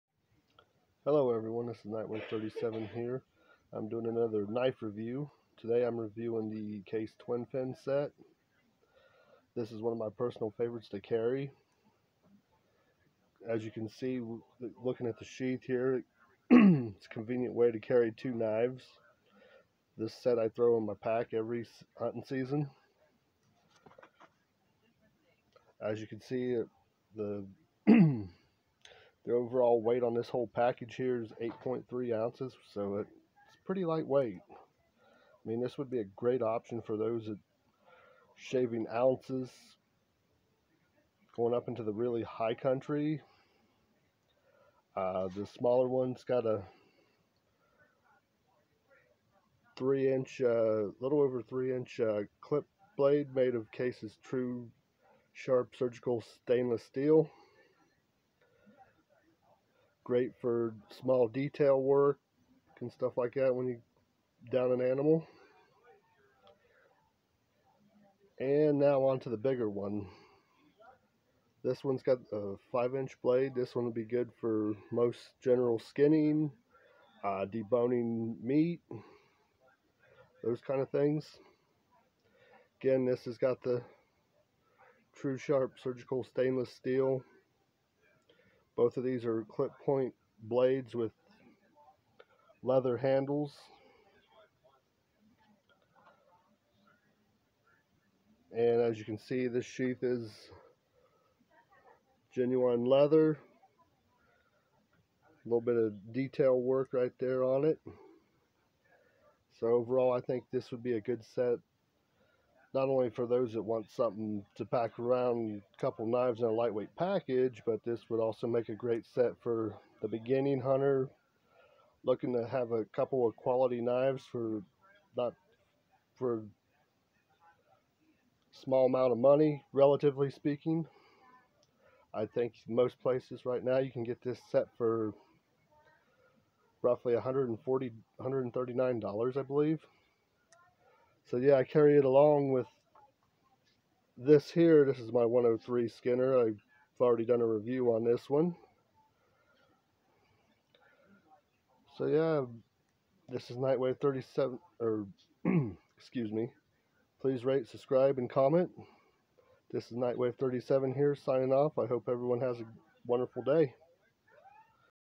Case Twin Finn Knives Set Review.
This is my review of the Case Twin Finn two knife set from W.R. Case & Sons. Inc.  These knives feature leather handles plus the leather sheath comes in this two knife combo. Both knives have Clip point blades made from Case's Tru-Sharp surgical stainless steel . The smaller knife has a 3.13 inch blade while the bigger one has a 5 inch blade.  These blades are very easy to sharpen and take an edge very well and this entire set is made in Bradford, Pennsylvania USA .Tru-Sharp Surgical Steel and Twin Finn are trademarks of W.R. Case&Sons Inc. I am an Amazon associate I earn from qualifying purchases on Amazon. Links are down below if you wish to purchase this product from Amazon.

Amazon Link To Case Twin Finn Two Knife Set